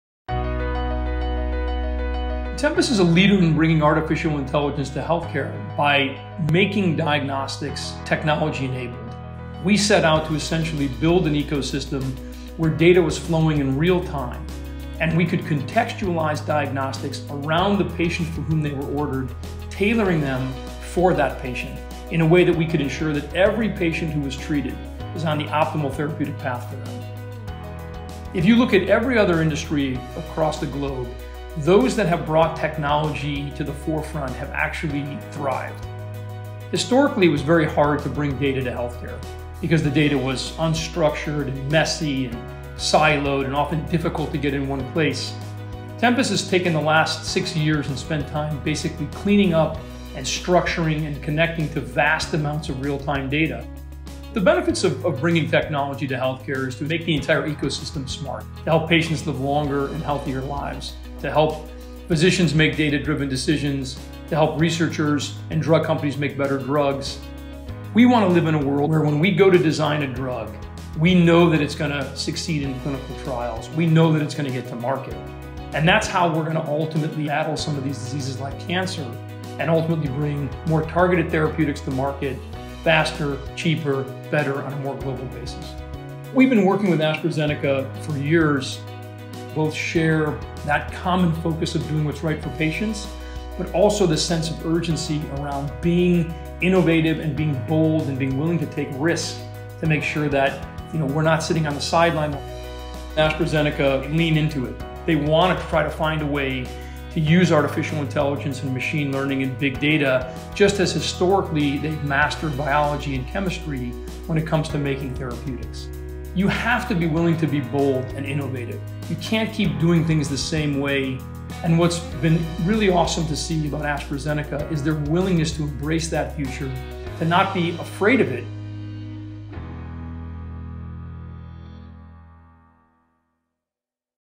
Digital Health R&D Collaborators: Tempus
While clinical trials capture important data, they ultimately represent a snapshot in time of a patient’s journey. Longitudinal, multimodal curated patient level data, like the kind that our partner Tempus has amassed, paints a more complete picture from which to draw rich insights – including looking at drivers of cancer biology, understanding how to better select patients for clinical trials or looking at analytics that can support drug discovery.

We’re pleased to collaborate with partners like Tempus to advance drug discovery and accelerate innovation for patients